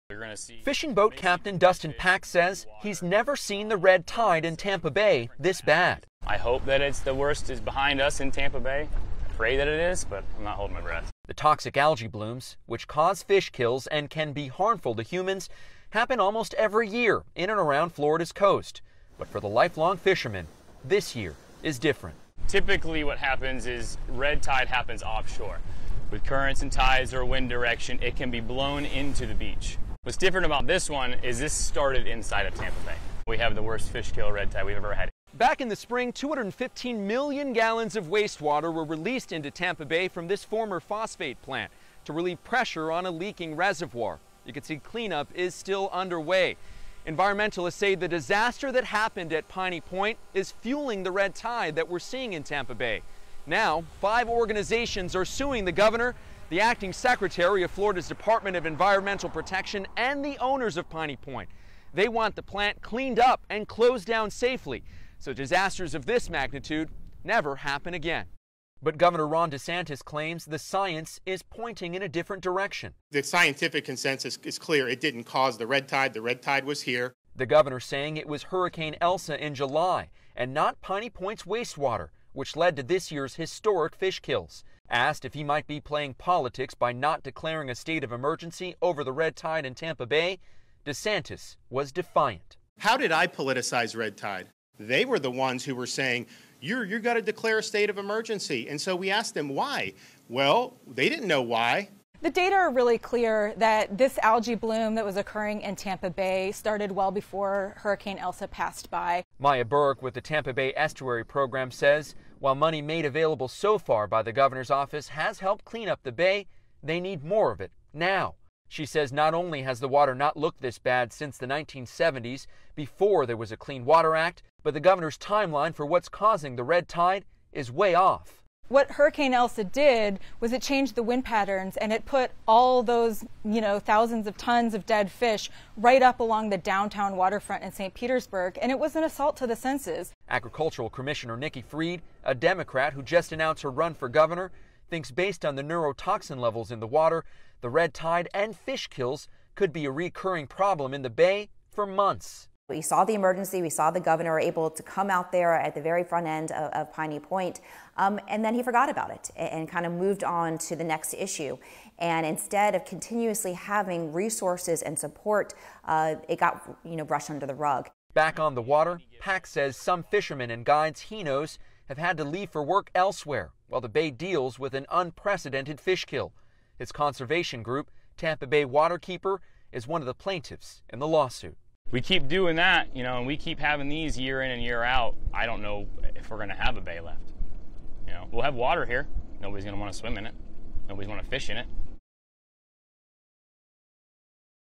ظاهرة محيّرة.. نفوق 600 طن من الأسماك بشكل غريب على شواطئ فلوريدا
في ظاهرة محيّرة نفق أكثر من 600 طن من الأسماك والأحياء البحرية في ولاية فلوريدا. وبينما تشير الدلائل الى أن ظاهرة المد الأحمر، وهو تكاثر للطحالب السامة، هي التي تسببت في قتل مئات آلاف الأسماك، أفاد نيك فالنسيا مراسل CNN أن حكومة فلوريدا وعلماء البيئة على خلاف حول السبب.